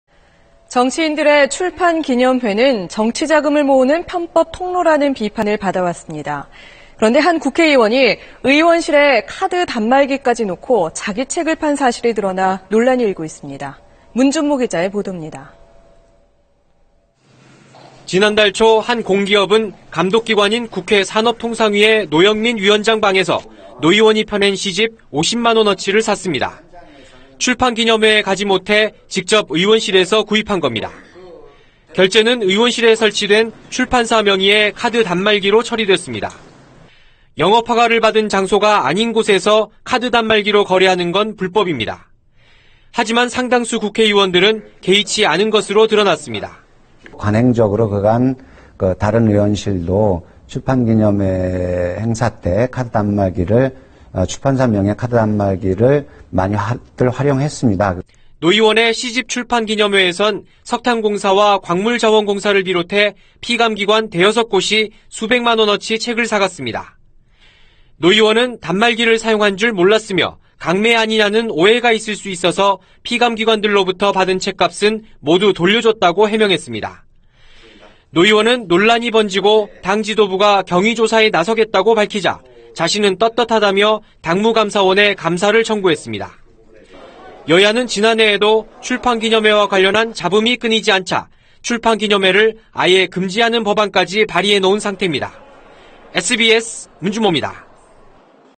의원실에 웬 카드 단말기?…대놓고 책 판매 논란 / SBS
정치인들의 출판기념회는 정치자금을 모으는 편법 통로라는 비판을 받아왔습니다. 그런데 한 국회의원이 의원실에 카드 단말기까지 놓고 자기 책을 판 사실이 드러나 논란이 일고 있습니다.

문준모 기자의 보도입니다.